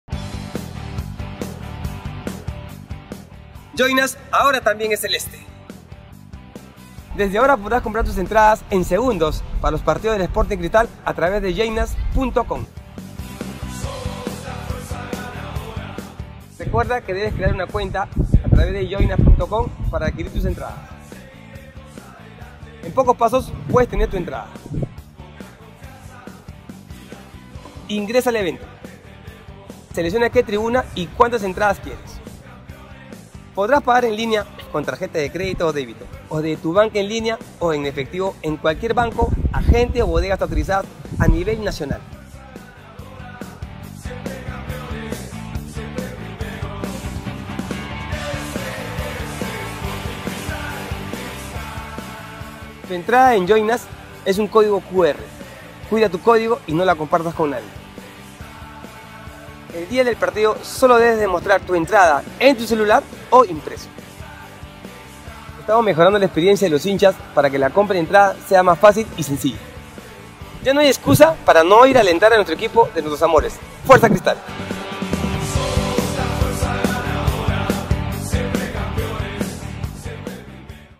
Cómo comprar tu entradas para el partido de Sporting Cristal.
¡Joinnus ahora es celeste!

El miércoles 30 de mayo el Club Sporting Cristal se enfrentará a su similar Club Alianza Lima por la fecha 3 del Torneo Apertura a las 8pm en el Estadio Nacional.

¡Ahora podrás comprar tu entrada en segundos en Joinnus.
DATO: El ingreso es obligatorio con DNI.